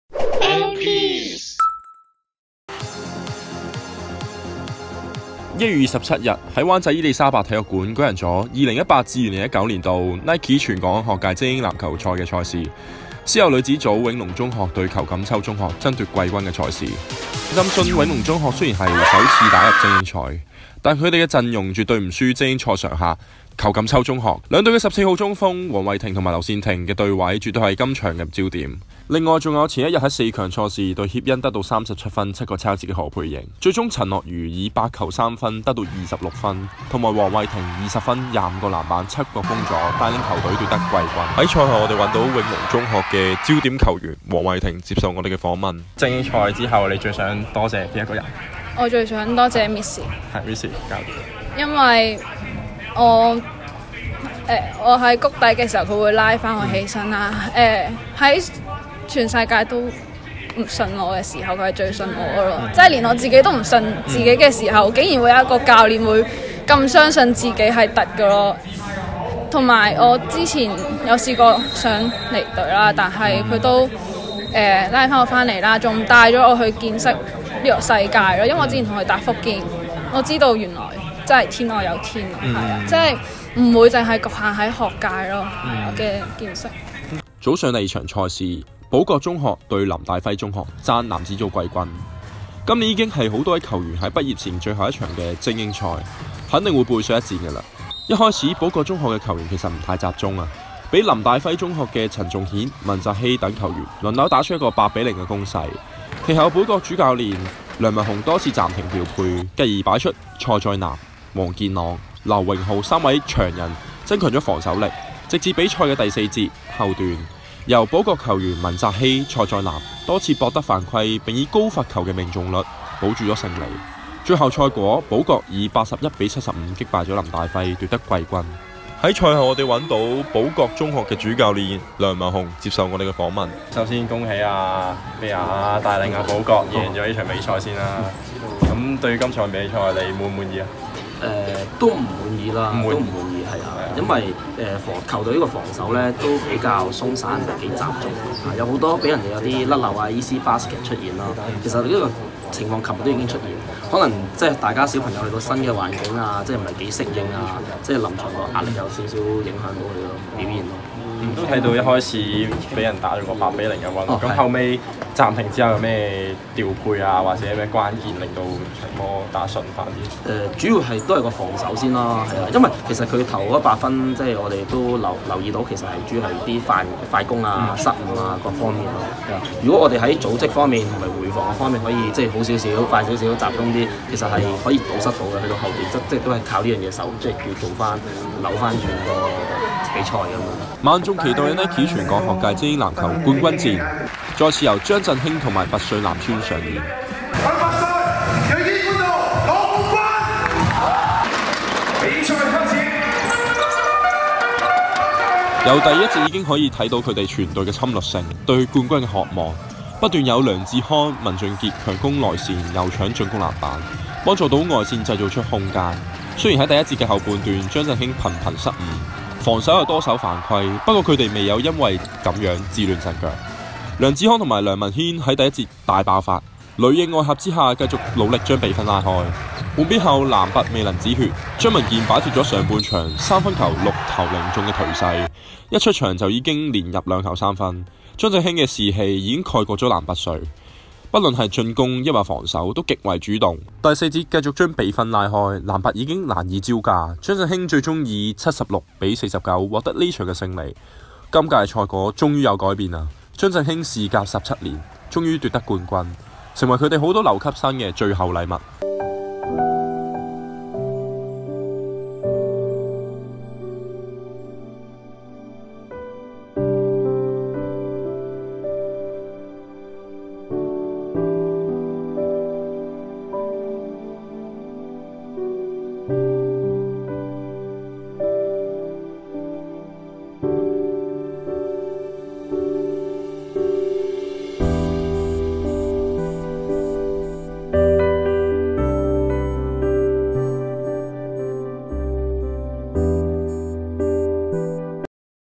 【香港學界籃球精英賽回顧】
上星期日於伊館開打的學界籃球精英賽，經過一輪比賽，最終冠、亞、季軍亦已塵埃落定。
現在，我們來回顧當天的四強與冠軍賽事吧！同場還有球員與教練的賽後訪問！(由於技術問題緣故，片段中沒有女子組的決賽片段。如果想重溫比賽內容，可以前往我們的專欄：